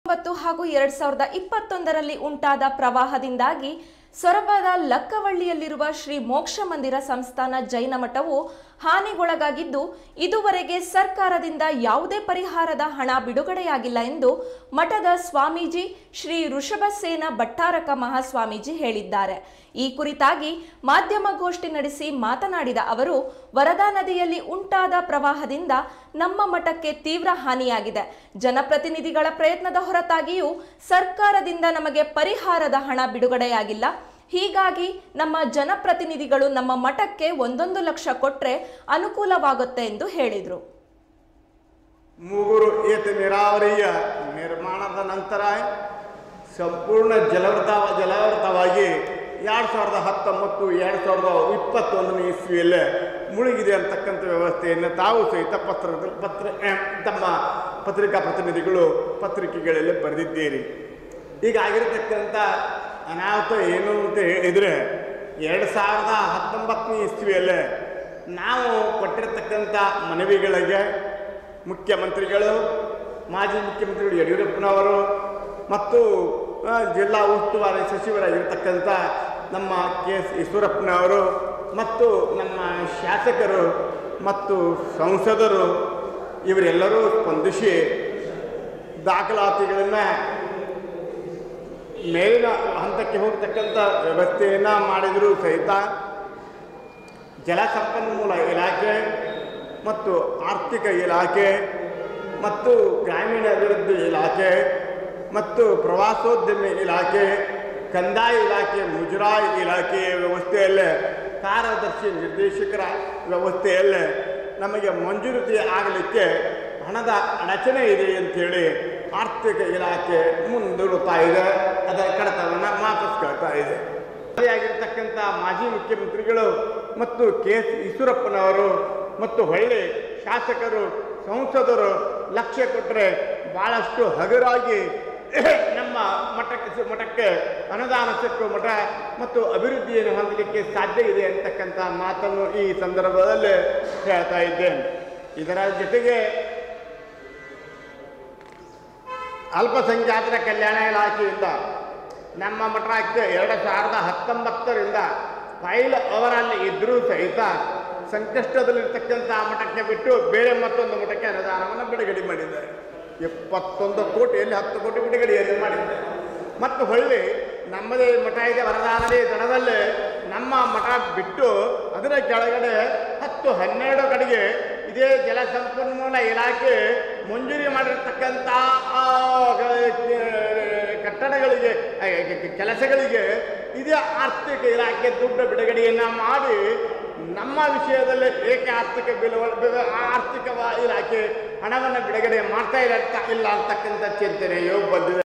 ಪ್ರವಾಹದಿಂದಾದ ಹಾನಿಗೆ ಇನ್ನೂ ಪರಿಹಾರ ಹಣ ಸಿಕ್ಕಿಲ್ಲ, ಶ್ರೀ ವೃಷಭಸೇನ ಭಟ್ಟಾರಕ ಮಹಾಸ್ವಾಮೀಜಿ
2019 ಹಾಗೂ 2021ರಲ್ಲಿ ಉಂಟಾದ ಪ್ರವಾಹದಿಂದಾಗಿ ಸೊರಬದ ಲಕ್ಕವಳ್ಳಿಯಲ್ಲಿರುವ ಶ್ರೀ ಮೋಕ್ಷ ಮಂದಿರ ಸಂಸ್ಥಾನ ಜೈನ ಮಠವು ಹಾನಿಗೊಳಗಾಗಿದ್ದು, ಇದುವರೆಗೆ ಸರ್ಕಾರದಿಂದ ಯಾವದೇ ಪರಿಹಾರದ ಹಣ ಬಿಡುಗಡೆಯಾಗಿಲ್ಲ ಎಂದು ಮಠದ ಸ್ವಾಮೀಜಿ ಶ್ರೀ ವೃಷಭಸೇನ ಭಟ್ಟಾರಕ ಮಹಾಸ್ವಾಮೀಜಿ ಹೇಳಿದ್ದಾರೆ. ಈ ಕುರಿತಾಗಿ ಮಾಧ್ಯಮಗೋಷ್ಠಿ ನಡೆಸಿ ಮಾತನಾಡಿದ ಅವರು, ವರದಾ ನದಿಯಲ್ಲಿ ಉಂಟಾದ ಪ್ರವಾಹದಿಂದ ನಮ್ಮ ಮಠಕ್ಕೆ ತೀವ್ರ ಹಾನಿಯಾಗಿದೆ. ಜನಪ್ರತಿನಿಧಿಗಳ ಪ್ರಯತ್ನದ ಹೊರತಾಗಿಯೂ ಸರ್ಕಾರಿಂದ ನಮಗೆ ಪರಿಹಾರದ ಹಣ ಬಿಡುಗಡೆಯಾಗಿಲ್ಲ. ಹೀಗಾಗಿ ನಮ್ಮ ಜನಪ್ರತಿನಿಧಿಗಳು ನಮ್ಮ ಮಠಕ್ಕೆ ಒಂದೊAದು ಲಕ್ಷ ಕೊಟ್ಟರೆ ಅನುಕೂಲವಾಗುತ್ತದೆ ಎಂದು ಹೇಳಿದರು.